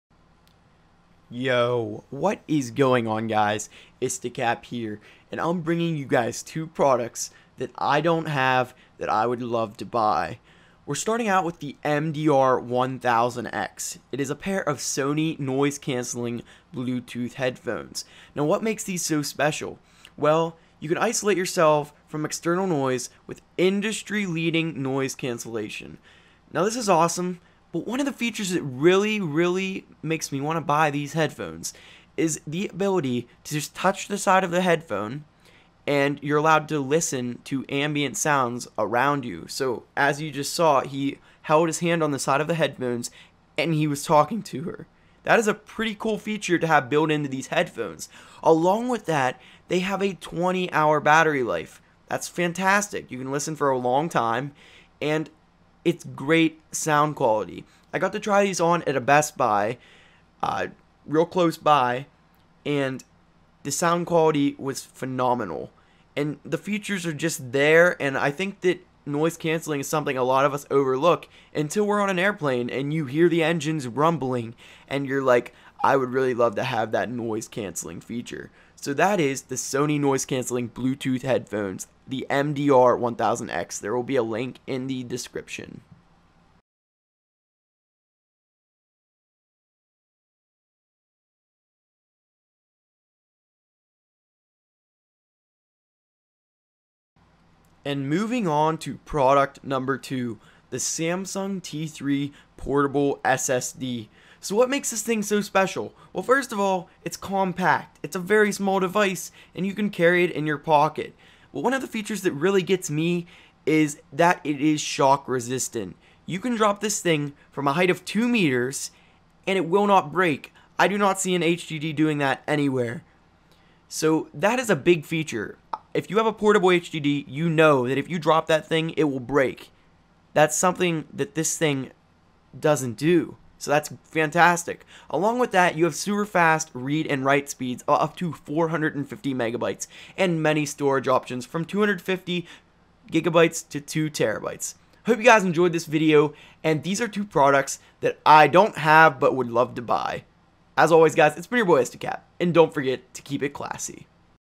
2 Products I Don't Have But Would Love to Buy! [Amazon Edition]
Links:
1. Sony Noise Cancelling, Bluetooth Headphones (MDR1000X)

2. Samsung T3 Portable SSD

Info:
Hi YouTube, These are two products which I don't have, but would love to buy. They seem like FANTASTIC products and they're made by very reputable companies.